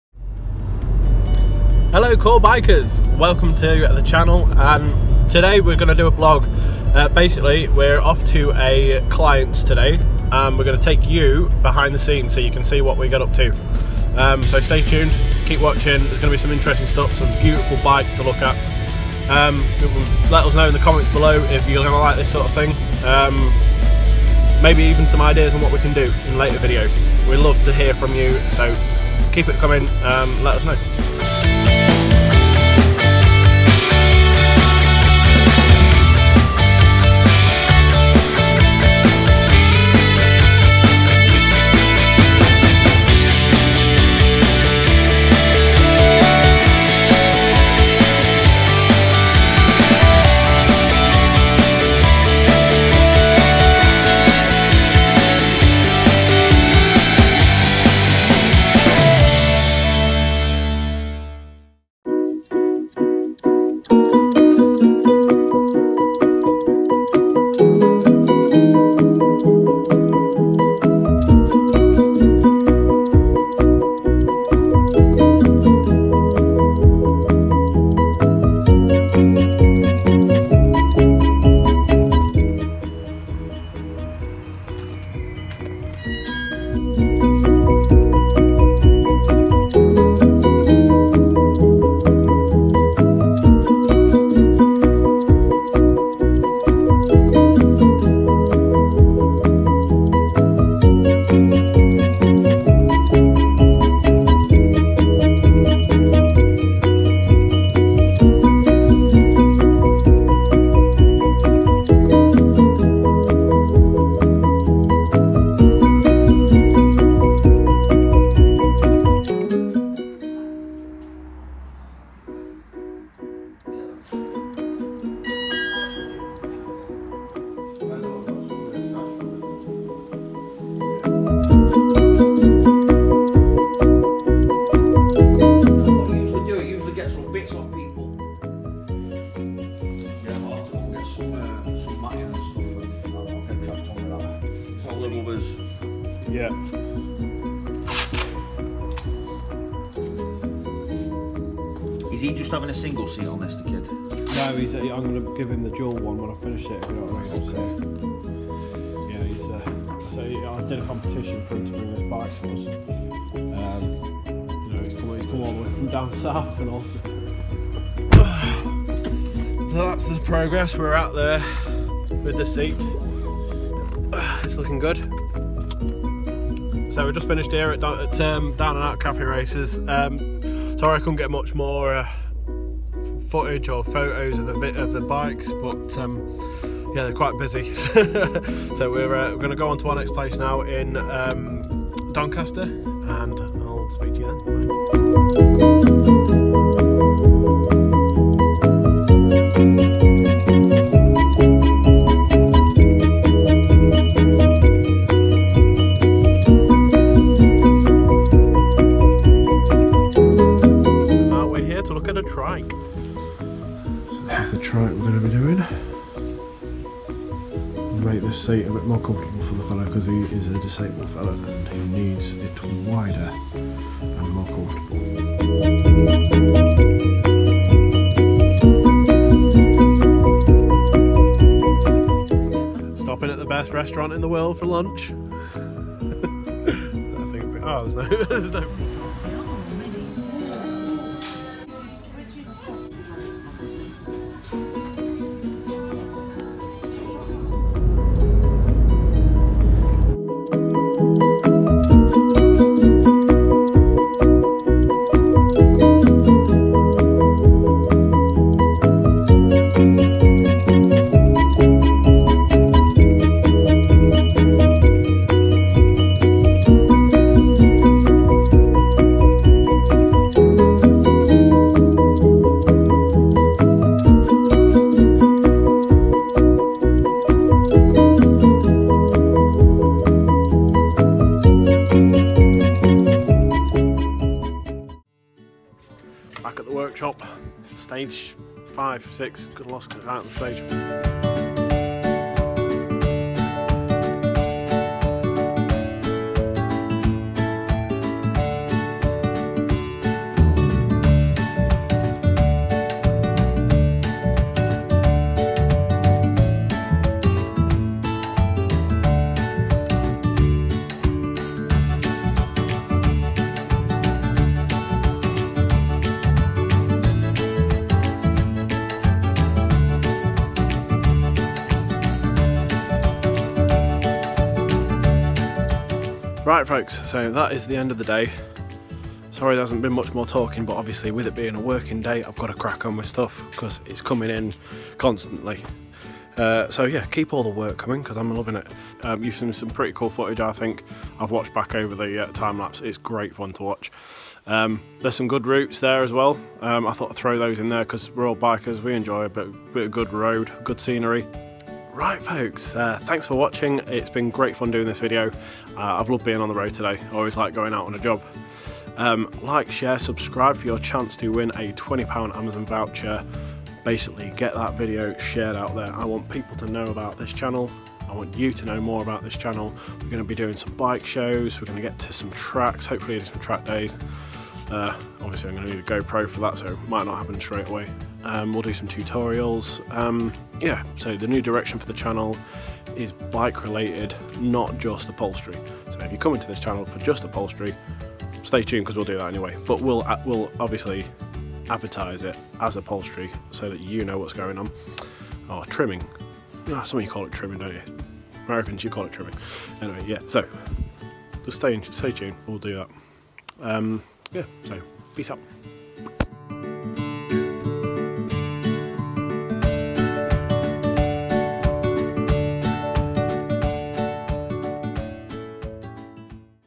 AWESOME ROAD TRIP TIME LAPSE!
Our first Video Log from behind the scenes at CORE motorcycle seating.

 We travel to Rotherham and visit Down & Out café racers to drop some seats off and see the progress of our first aftermarket seat!

Featured roads.. Till Bridge lane Lincolnshire, A631 between Rotherham and Lincoln!

watch to the end to get in on the reward!

links to our facebook and instagram pages can be found there!